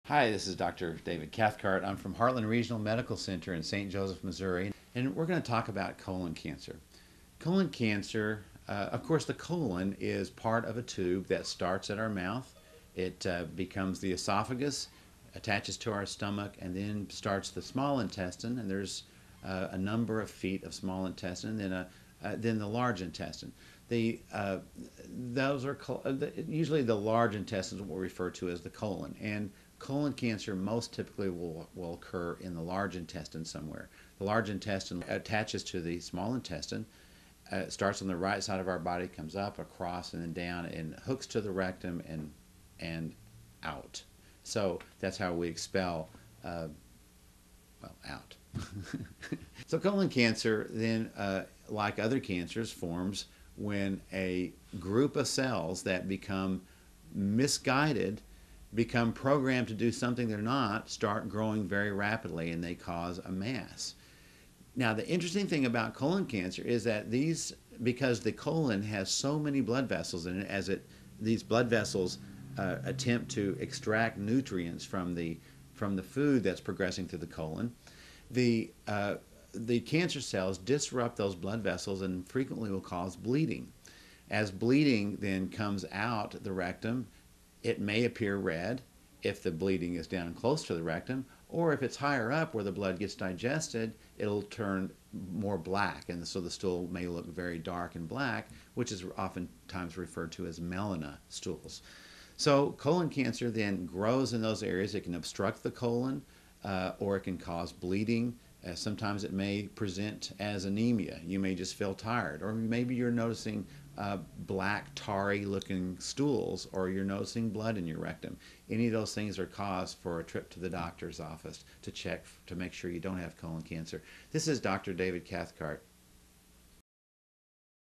Cancer Facts : How Does Colon Cancer Affect the Body?
Colon cancer can affect the body by causing anemia, tiredness, black or tarry stool and blood in the rectum. Learn how colon cancer cells leach nutrients from food going through the colon with information from a doctor in this free video on cancer.

Bio: Dr. David Cathcart specializes in occupational medicine and has an in-depth knowledge of cancer, as well as experience dealing with cancer patients and treatment for multiple years.